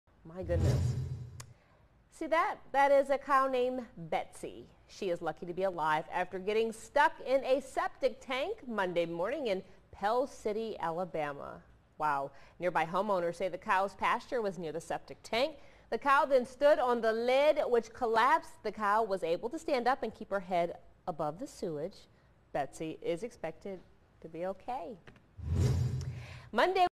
Cow Rescued from Septic Tank
PELL CITY, Ala. (WPMI) A cow is in good health after falling into a septic tank Monday morning. The rescue took about five hours.

Betsy the cow collapsed into a 1,000 gallon tank that was 80 percent full of sewage. She was able to stand up and keep her head above the sewage.

A septic tank company was able to pump the waste out, then they sprayed the sewage off  Betsy.

Crews were able to float Betsy out of the tank with water. They then got straps under her and assisted her out of the tank.

The septic tank was not damaged.

(Photos courtesy of Capt. Tim Debasky, Pell City Fire Department)